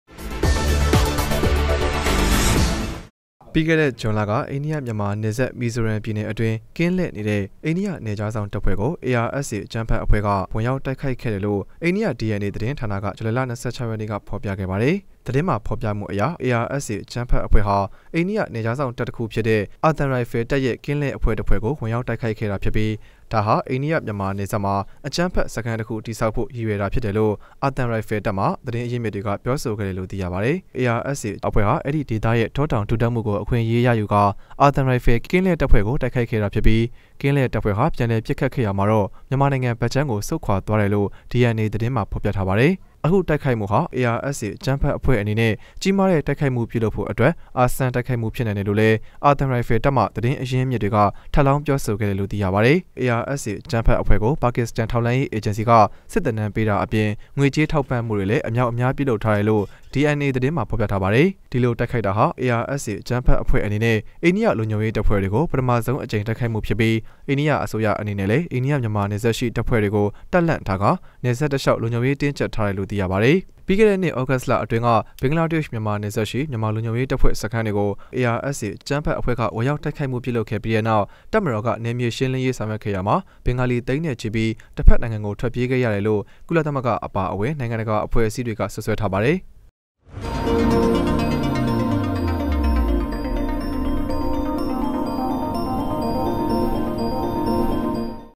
အိႏၵိယနယ္ျခားေစာင့္တပ္ကုိ ARSAအၾကမ္းဖက္အဖြဲ႕ကတုိက္ခုိက္ခဲ့ဟု
ၿပီးခဲ့တဲ့ဇြန္လကအိႏိၵယျမန္မာနယ္စပ္မီဇုိရမ္ျပည္နယ္အတြင္းကင္းလွည့္ေနတဲ့အိႏိၵယနယ္ျခားေစာင့္တပ္ဖြဲ႕ ကုိARSAအၾကမ္းဖက္အဖြဲ႕က ၀င္ေရာက္တိုက္ခုိက္ခဲ့တယ္လုိ႔ အိႏၵိယ DNA သတင္းဌာနကဇူလုိင္လ ၂၆ ရက္ေန႔ကေဖာ္ျပခဲ့ပါတယ္။